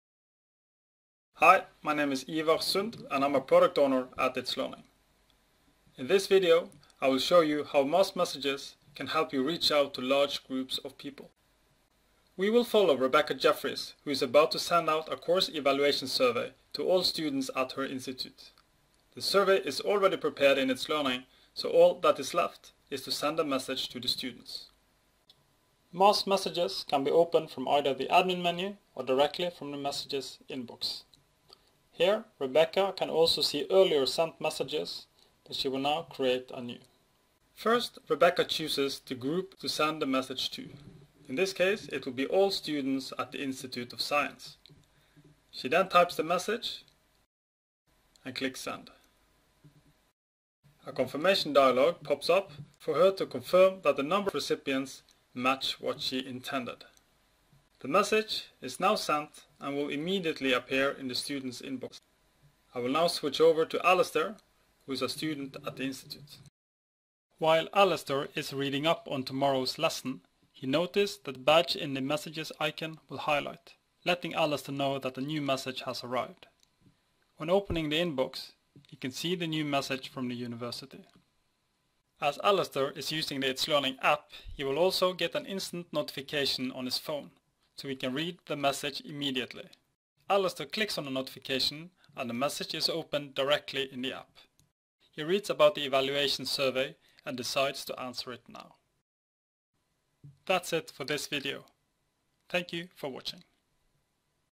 itslearning Tutorial: Mass Messaging
Send an instant message to large groups of people (everyone in a hierarchy) with the itslearning Mass Messaging feature, new with Back to School 2019.